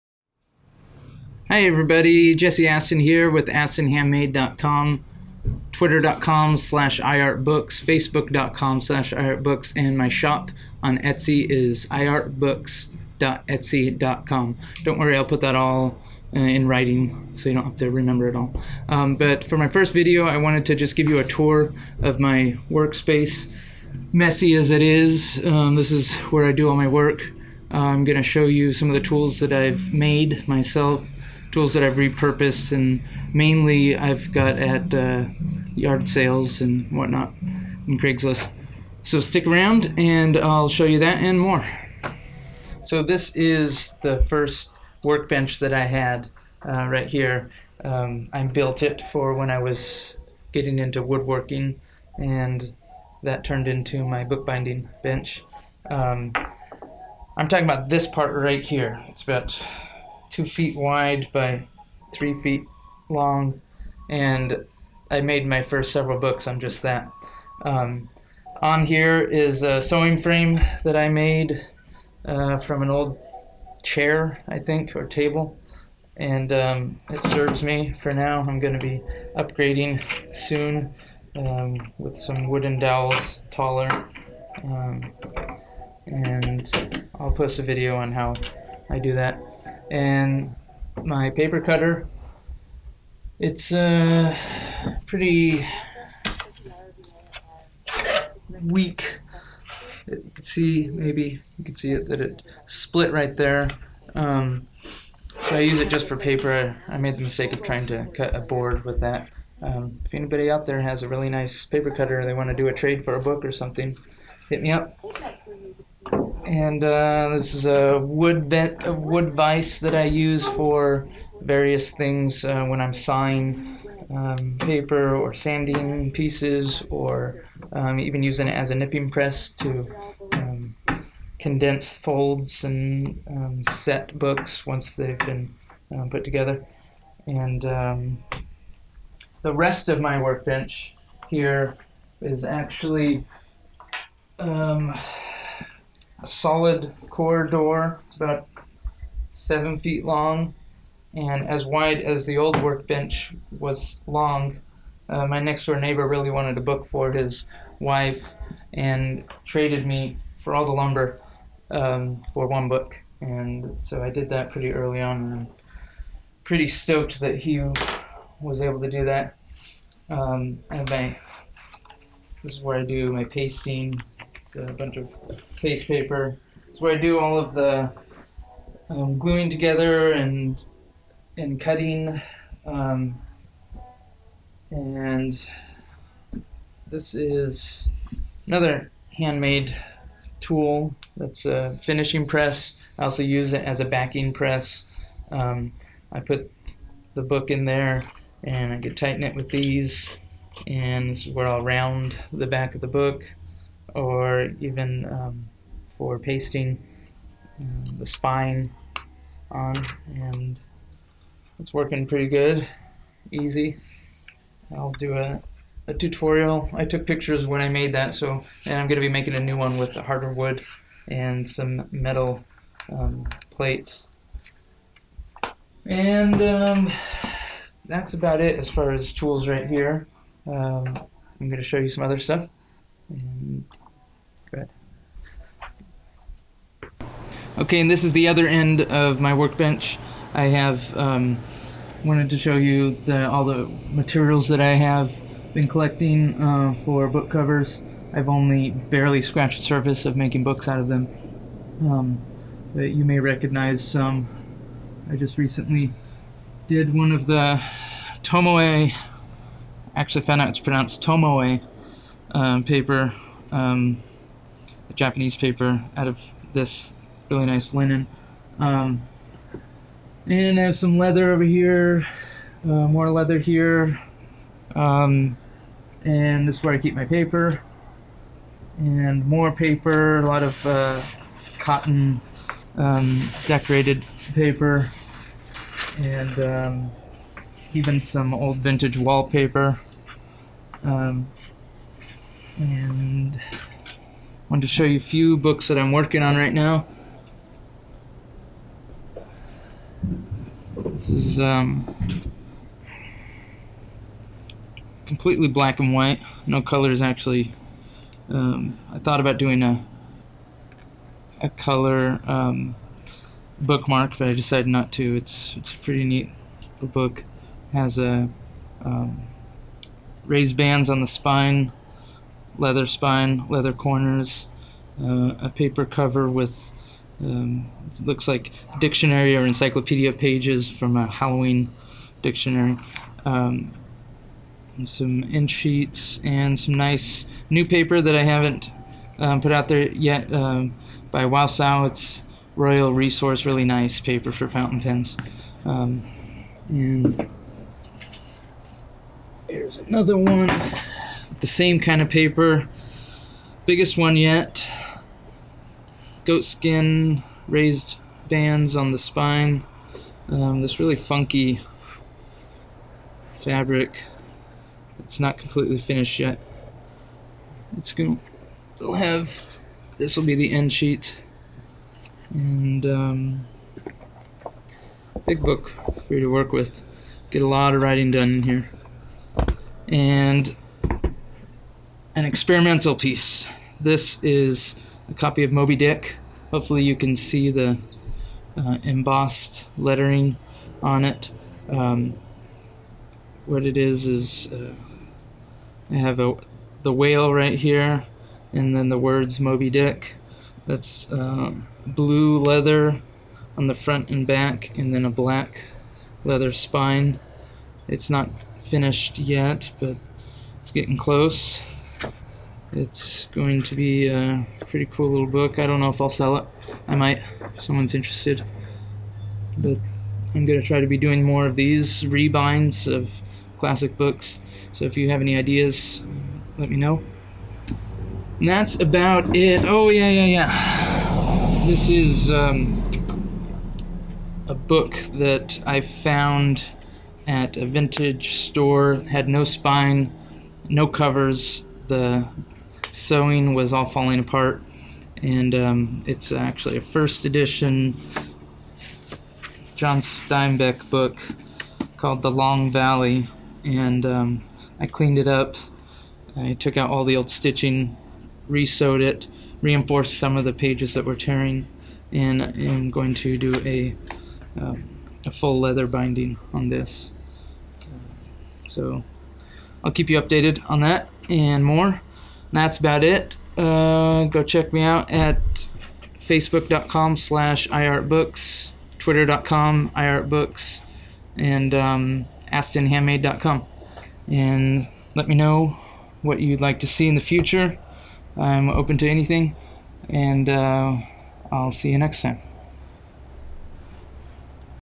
A Tour of my Bookbinding Workspace
A quick tour of my workshop for my bookbinding. I show some handmade tools, repurposed tools, the fabrics and leather which I use.